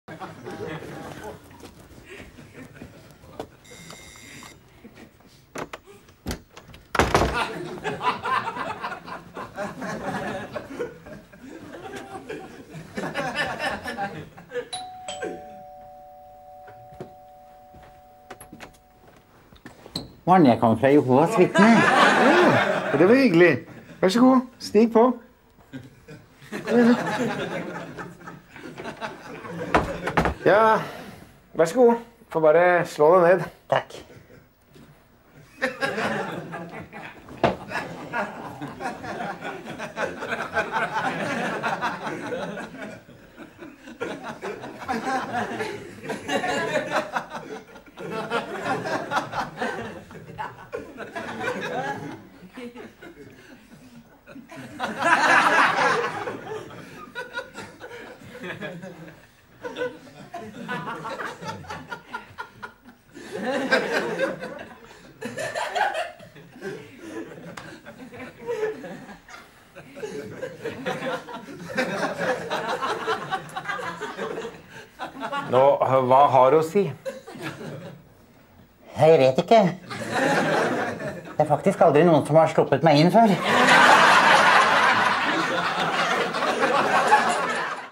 Hjemmebesøk fra Jehovas Vitne
Heldigvis er det få som vet hva som skjer om du slipper dem innenfor dørkarmen, men her har du svaret.